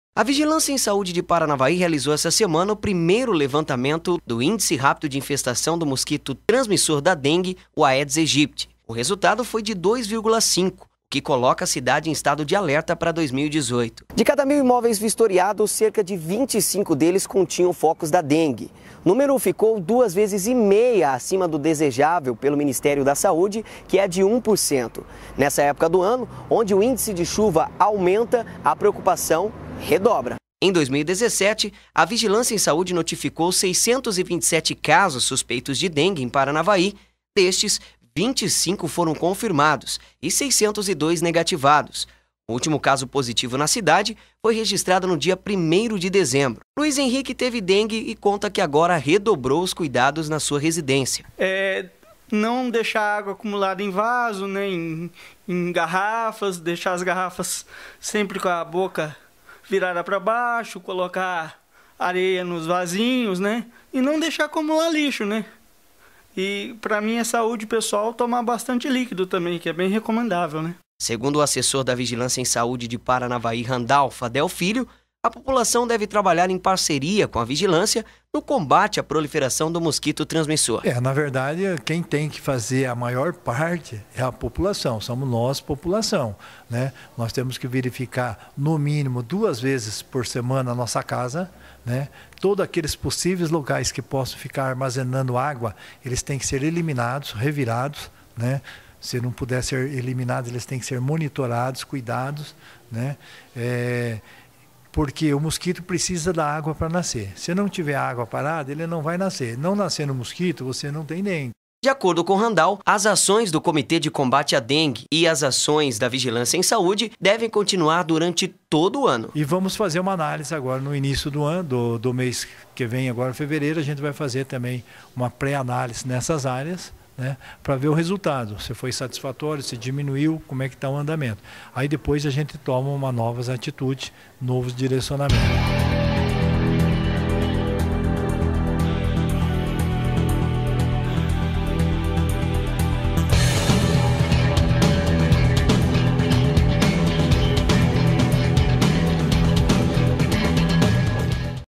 Dengue em Paranavaí
A vigilância em saúde de Paranavaí lançou essa semana o primeiro Índice de Infestação do mosquito transmissor da dengue o Aedes Aegypti, segundo dados da vigilância, o índice esta superior ao desejável pelo ministério da saúde. Acompanhe na reportagem.